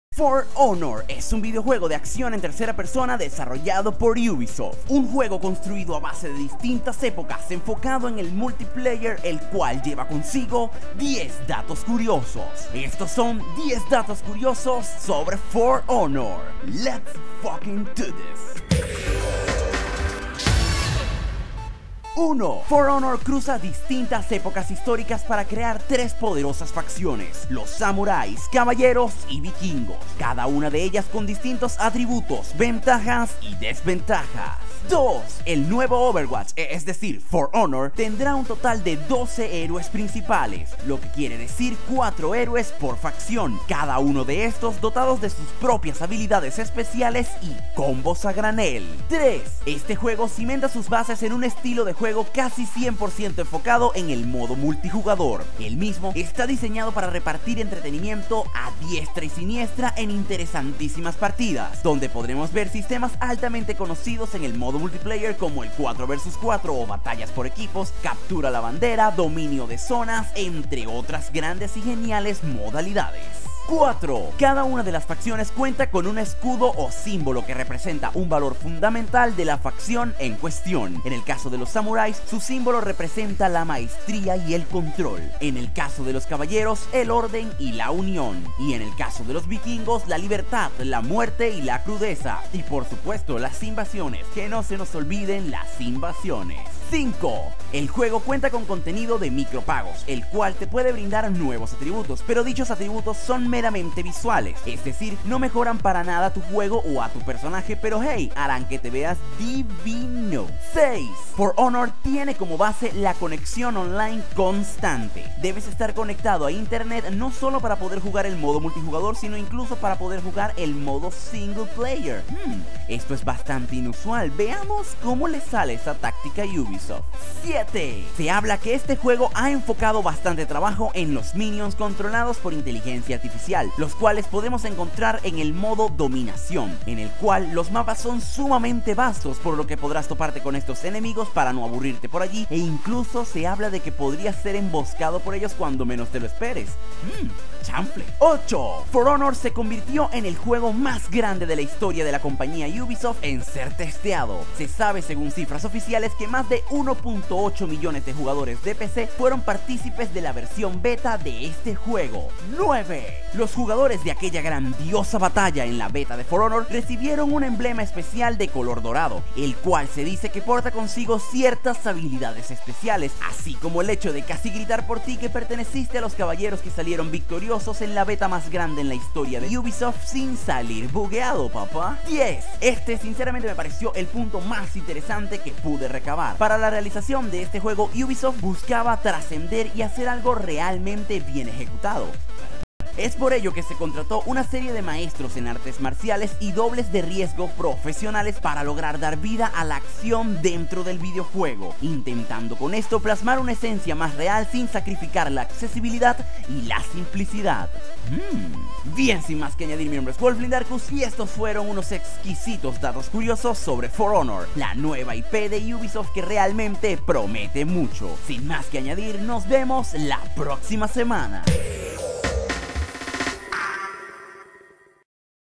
10 datos curiosos sobre For Honor
For Honor tiene todo lo que un gamer podría esperar. Caballeros, Vikingos y Samurais -y eso sin contar la gran amenaza que les espera- y distintas modalidades para que todos los gamers encuentren la que más les gusta.

Pero For Honor va más allá. Para descubrir que tanto, chequea sus 10 datos más interesantes en este video.

Creador de el vídeo: Wolfin

Visita nuestra web para estar al tanto de nuestro servicio y las mejores noticias sobre el mundo de los videojuegos.